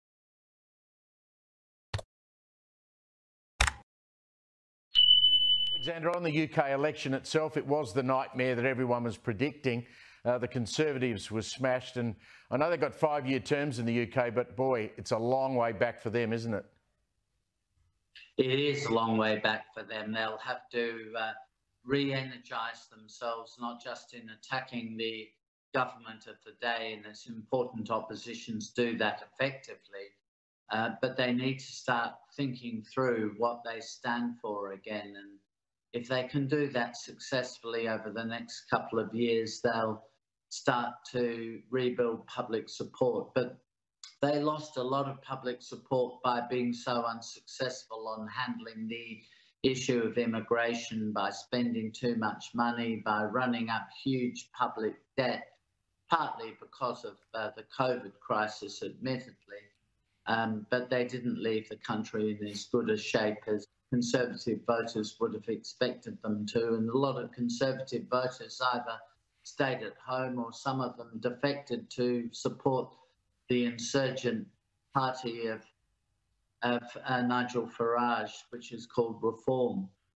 The Conservatives need to start thinking through what they stand for again.
Former Australian foreign minister Alexander Downer has called on the UK Conservative Party to “start thinking” through “what they stand for” following their election loss last week.